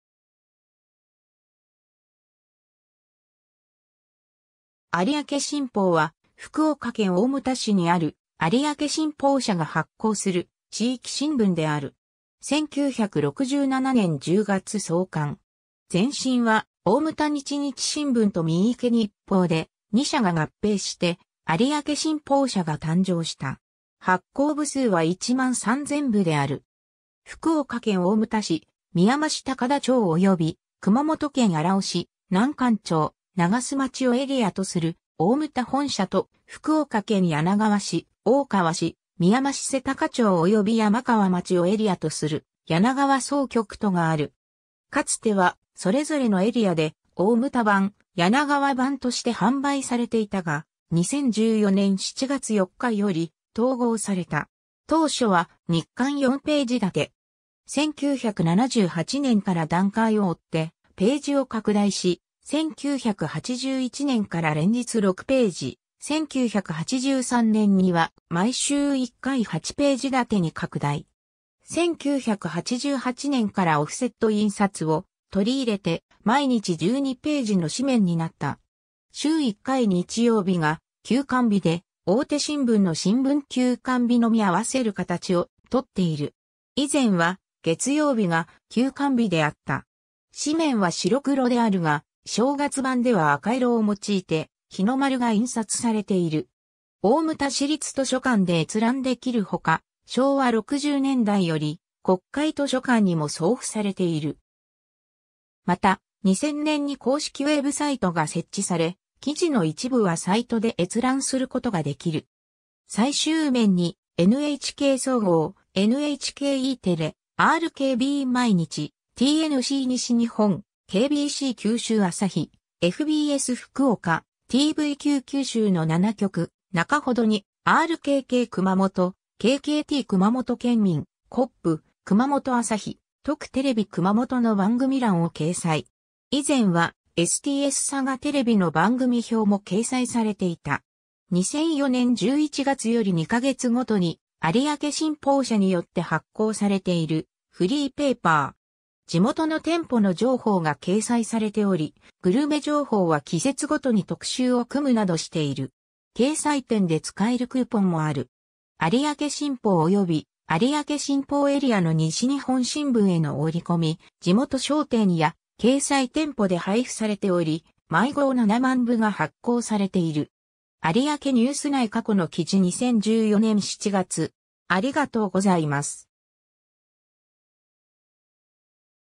有明新報
有明新報（ありあけしんぽう）は福岡県大牟田市にある、有明新報社が発行する地域新聞である。
 1967年（昭和42年）10月創刊。
前身は大牟田日日新聞と三池日報で、2社が合併して有明新報社が誕生した。
発行部数は1万3000部である。
 福岡県大牟田市・みやま市高田町及び熊本県荒尾市・南関町・長洲町をエリアとする「大牟田本社」と、福岡県柳川市・大川市・みやま市瀬高町及び山川町をエリアとする「柳川総局」とがある。
かつてはそれぞれのエリアで「大牟田版」「柳川版」として販売されていたが、2014年（平成26年）7月4日より統合された。
 当初は日刊4ページ立て。
1978年（昭和53年）から段階を追ってページを拡大し、1981年（昭和56年）から連日6ページ、1983年（昭和58年）には毎週1回（基本的に金曜日）8ページ立てに拡大。
1988年（昭和63年）からオフセット印刷を取り入れて毎日12ページの紙面になった。
週1回日曜日が休刊日で、大手新聞の新聞休刊日のみ合わせる形をとっている。
以前は月曜日が休刊日であった。
紙面は白黒であるが、正月版では赤色を用いて日の丸が印刷されている。
 大牟田市立図書館で閲覧できるほか、昭和60年代より国会図書館にも送付されている。
また、2000年に公式ウェブサイトが設置され、記事の一部はサイトで閲覧することができる。
 最終面にNHK 総合（福岡局）、NHK Eテレ、RKB毎日、TNC 西日本、KBC 九州朝日、FBS 福岡、TVQ 九州の7局、中ほどにRKK 熊本、KKT くまもと県民、KAB 熊本朝日、TKU テレビ熊本の番組欄を掲載。
以前はSTSサガテレビの番組表も掲載されていた。
 2004年11月より2ヶ月ごとに有明新報社によって発行されているフリーペーパー。
地元の店舗の情報が掲載されており、グルメ情報は季節ごとに特集を組むなどしている。
有明新報および有明新報エリアの西日本新聞への折込、地元商店や掲載店舗で配布されており、毎号70,000部が発行されている。
 ^ ariake-news内*過去の記事* 2014年7月（archive版）